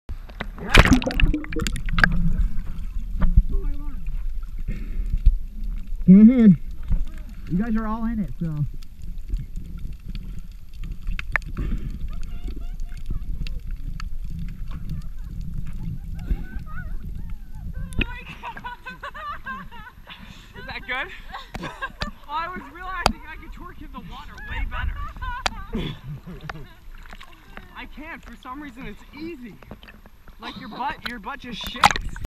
Snorkel Twerking
Cinnamon Bay, St John.  Niklas wanted to show off how much easier underwater twerking is while snorkeling the Cinnamon Cay.